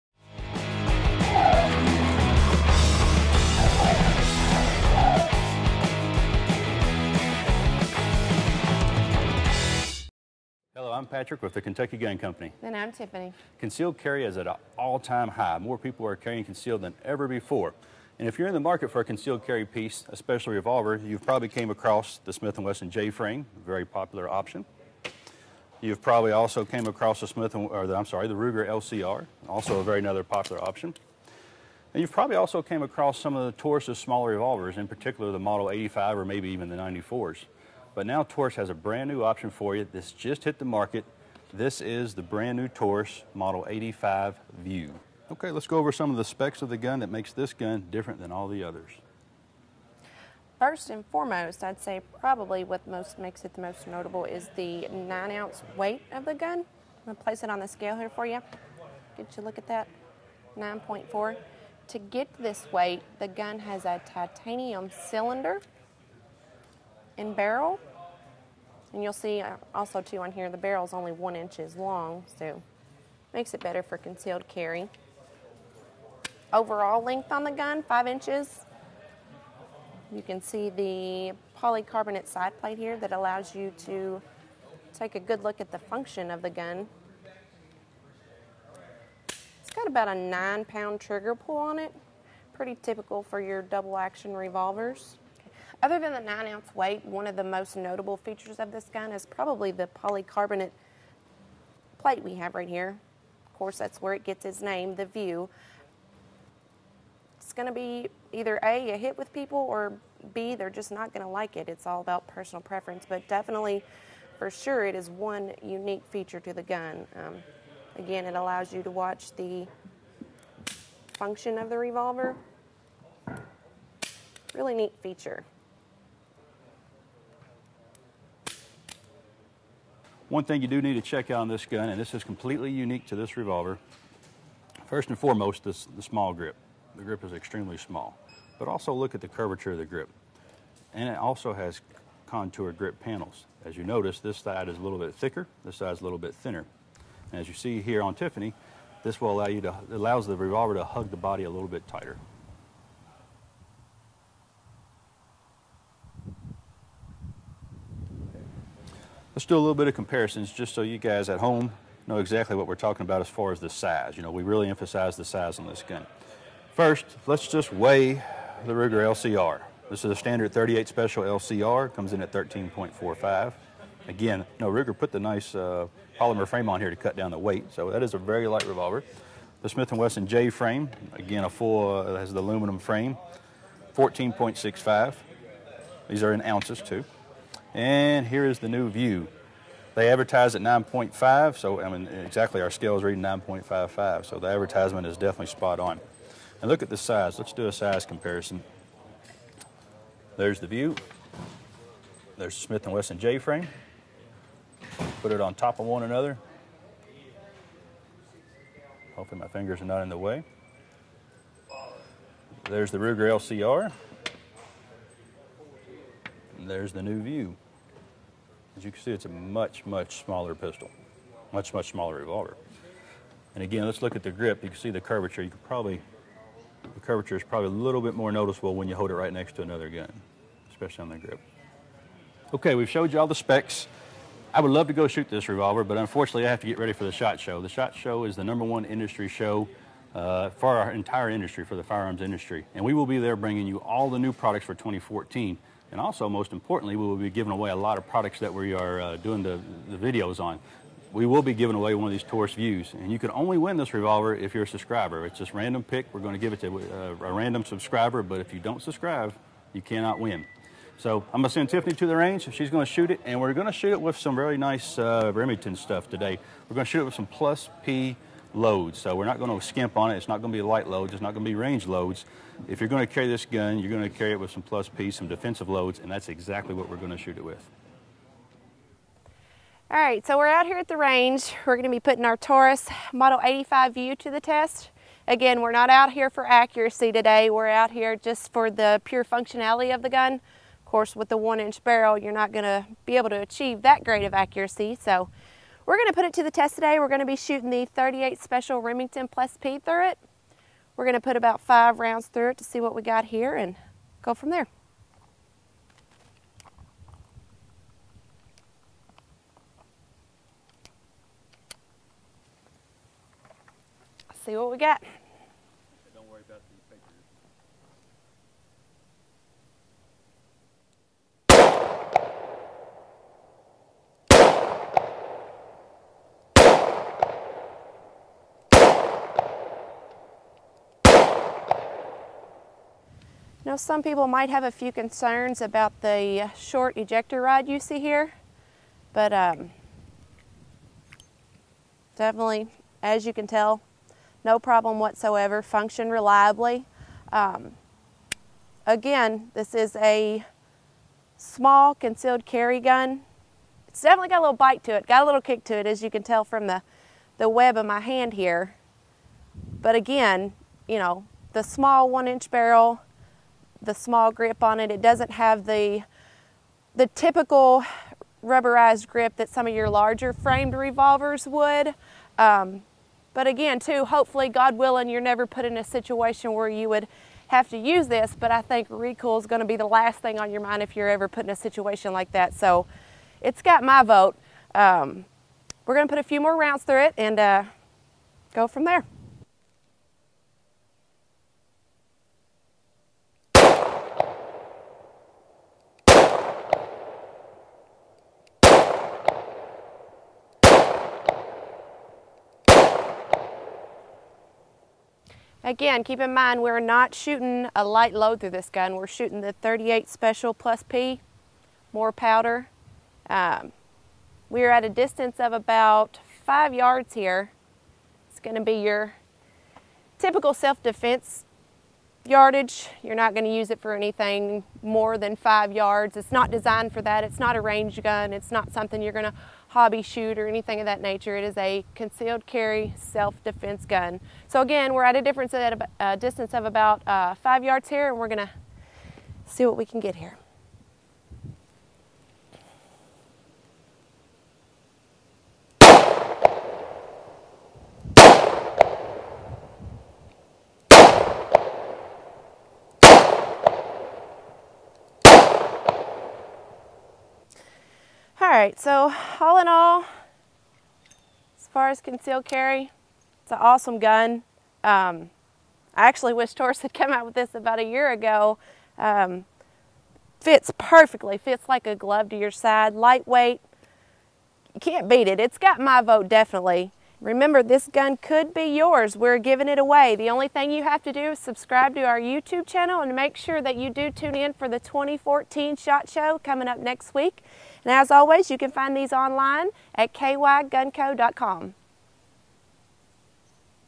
Taurus 85 View SHOT Show 2014 Preview
Patrick and Tiffany get their hands on the new Taurus Model 85 View and put it right to the test!  As you can see, this is new gun is great for concealed carry because of its unique size and it weighs in at only 9.5 ounces!!

Taurus does not recommend the use of +P ammunition with this revolver!!